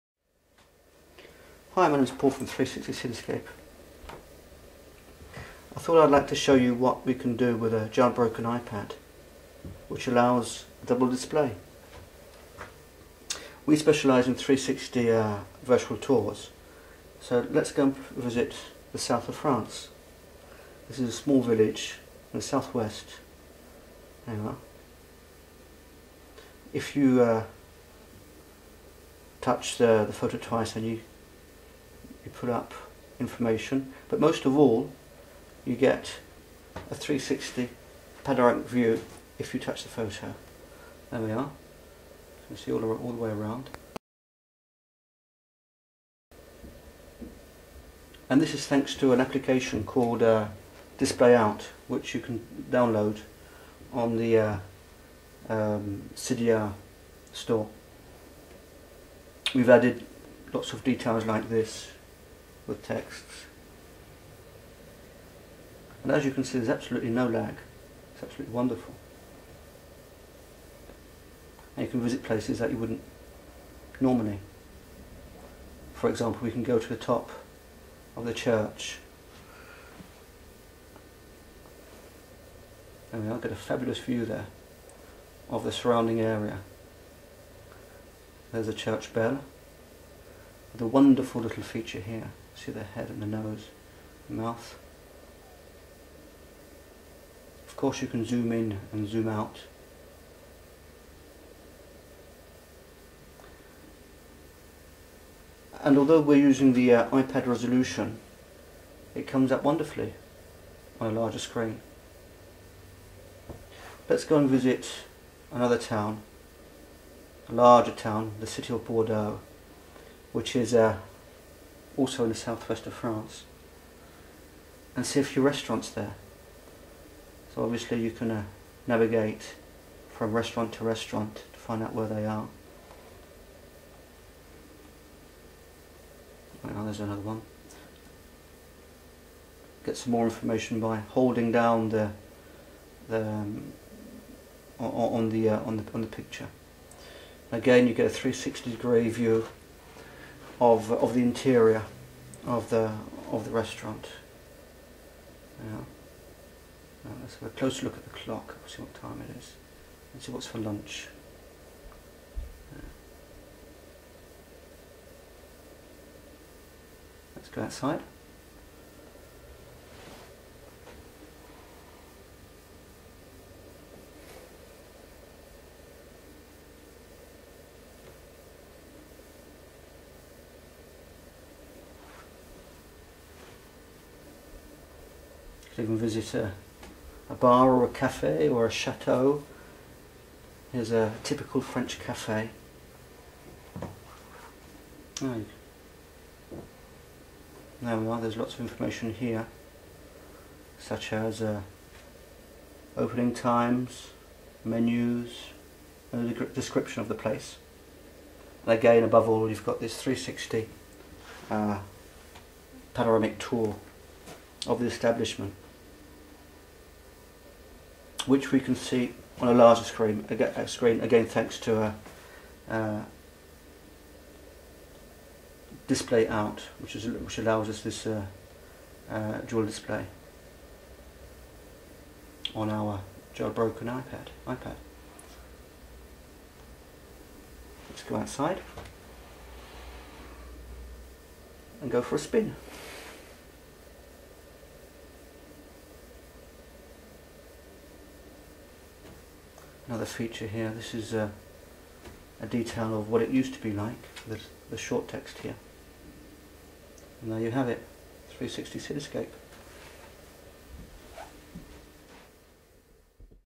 How to display an iPad interface on a TV screen using DisplayOut ?
This clip shows you an easy way to output an iPad1 interface to a larger screen.